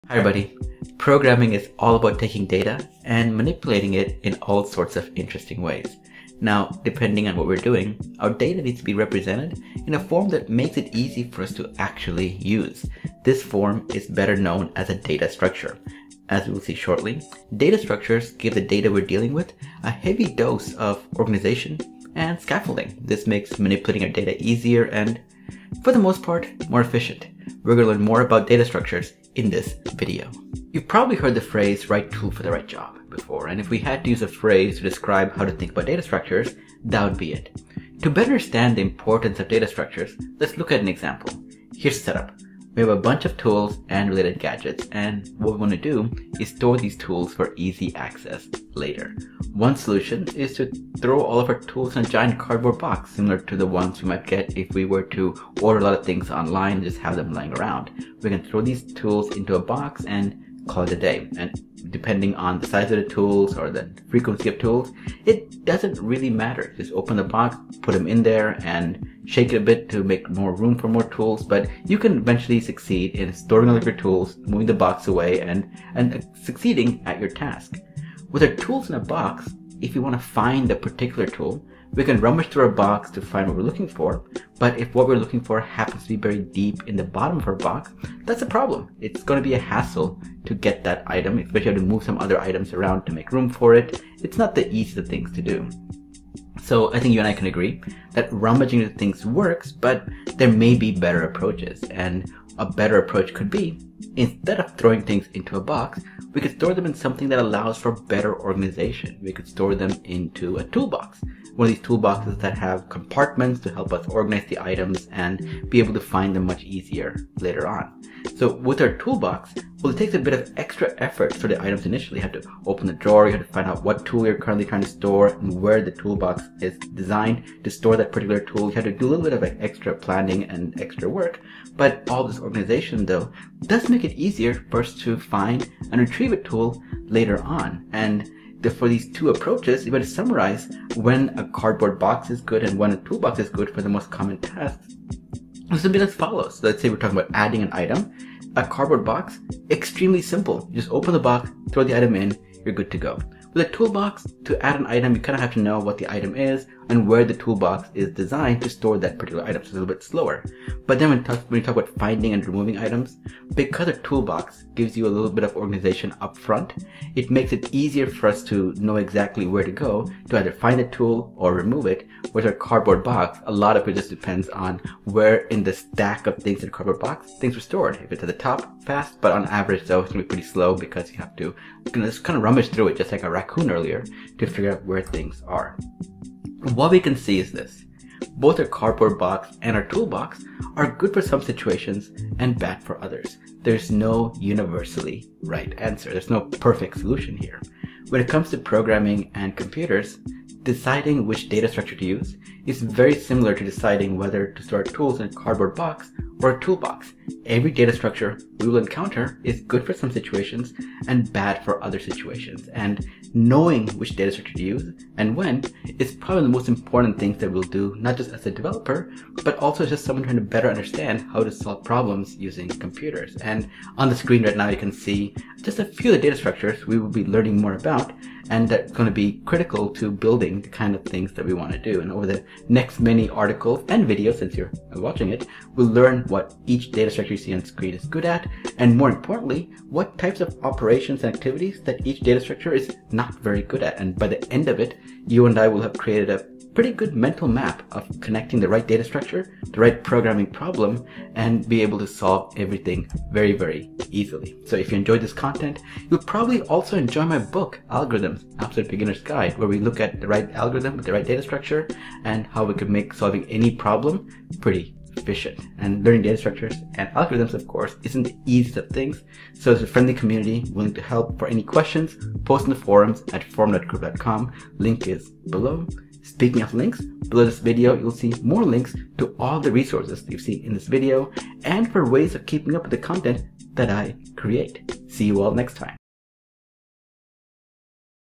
Introduction to Data Structures and why they matter! 🎯
Data structures are the fundamental building blocks of modern-day programming. Learn more about what they are and why they matter.